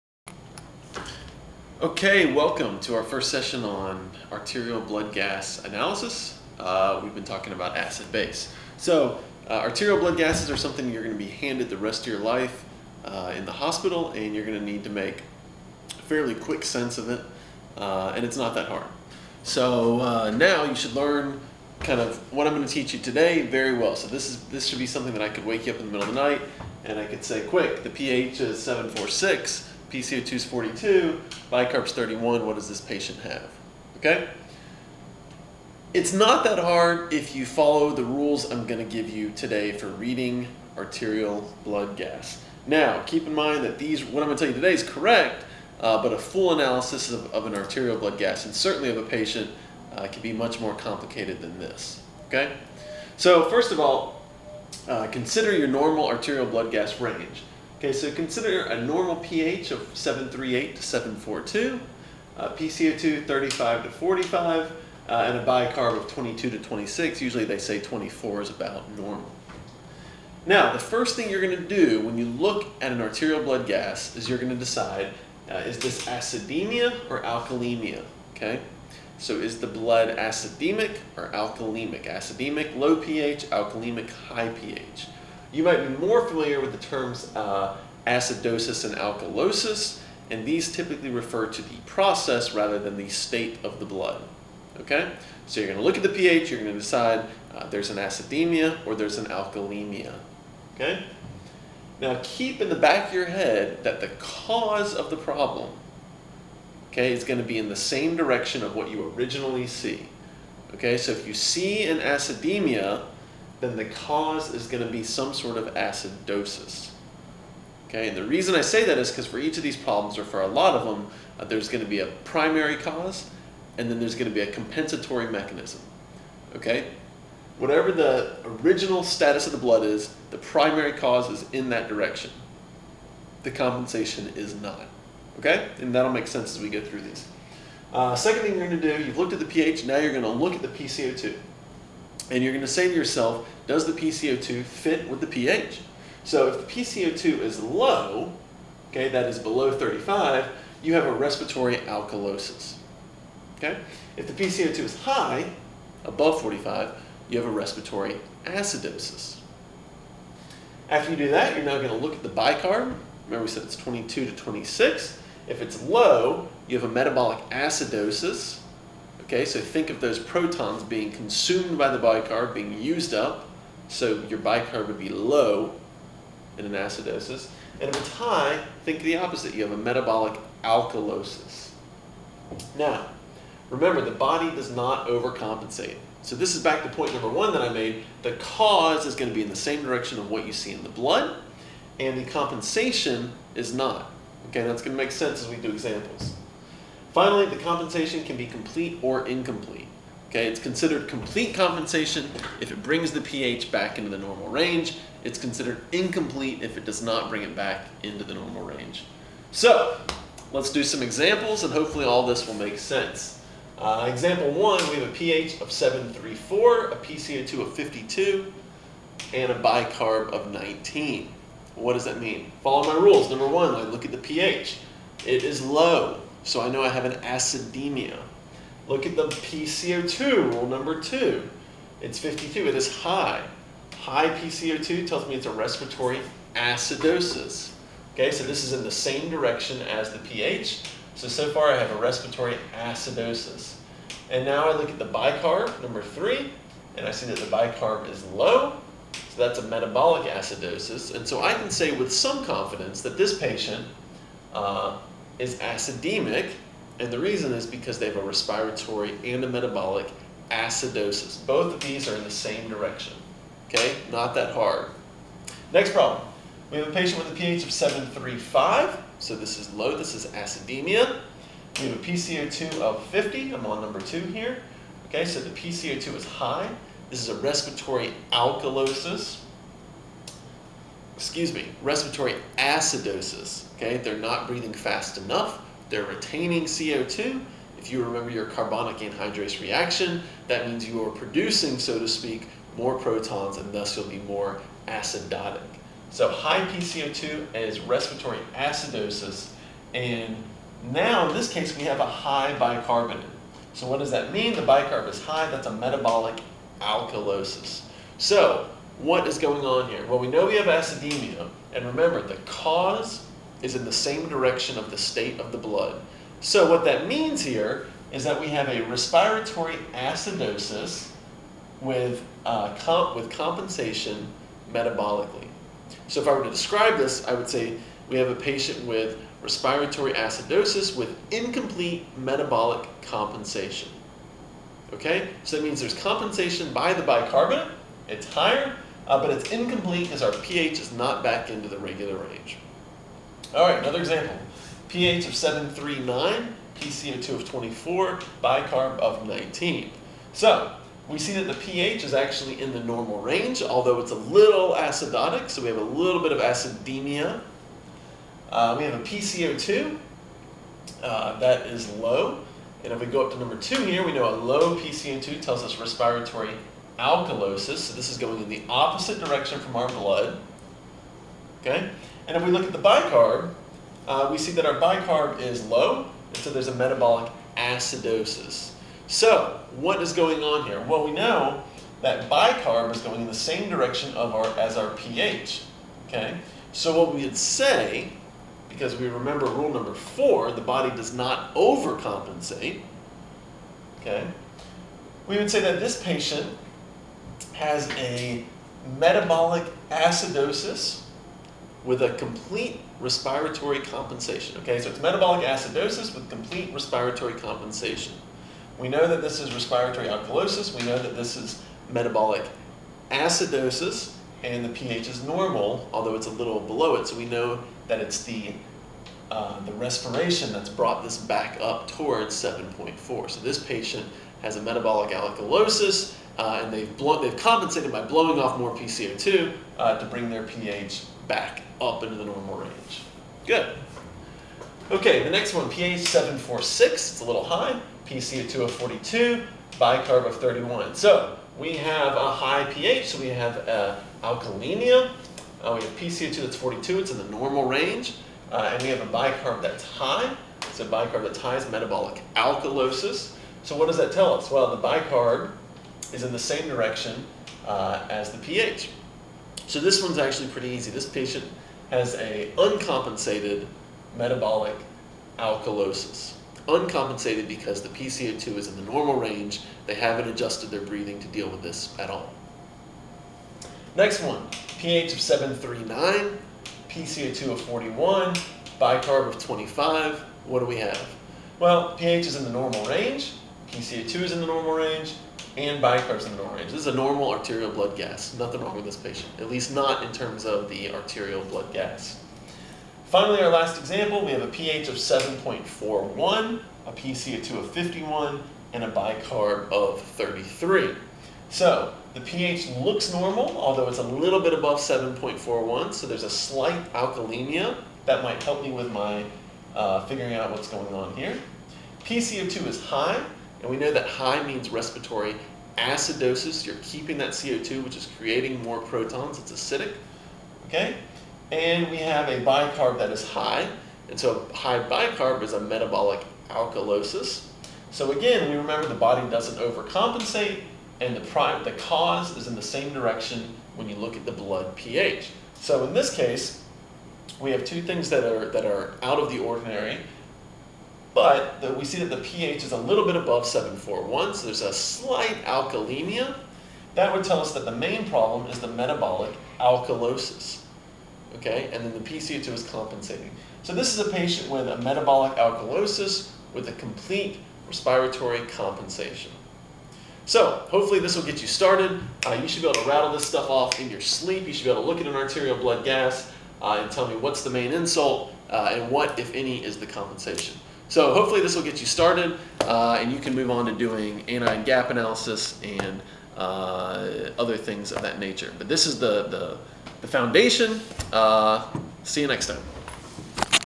ACID BASE 1: Arterial Blood Gas (ABG) Interpretation; Acid-Base
Learn from Dr. G how to quickly analyze an arterial blood gas, something that most doctors will need to do for the rest of their careers.

Email questions to Dr G at YOUTUBE channel.